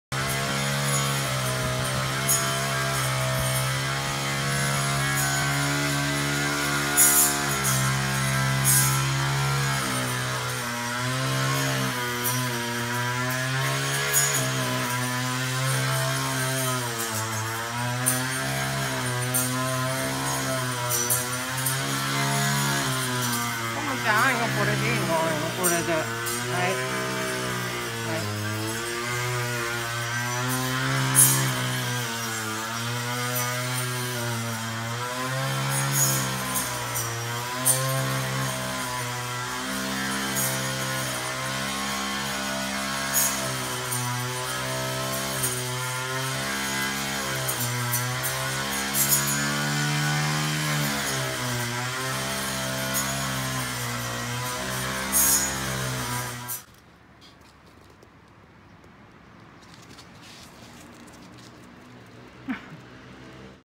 みんなから注意され、座り方を改めたこむぎ【Akita-inu】Correct sitting
赤毛秋田犬こむぎ１歳１カ月

結局、変な座り方です😅